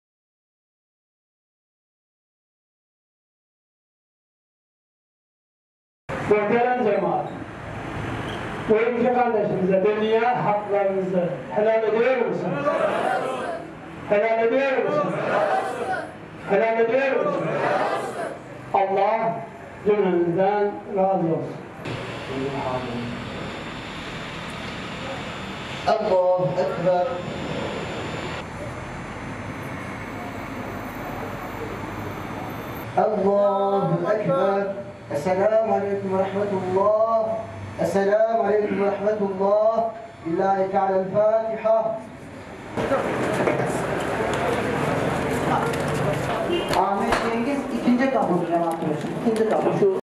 Yeşilçam'ın emektarı "Artist Ahmet" vefat etti
Yeşilçam oyuncularından "Artist Ahmet" lakaplı Ahmet Cengiz 70 yaşında hayatını kaybetti.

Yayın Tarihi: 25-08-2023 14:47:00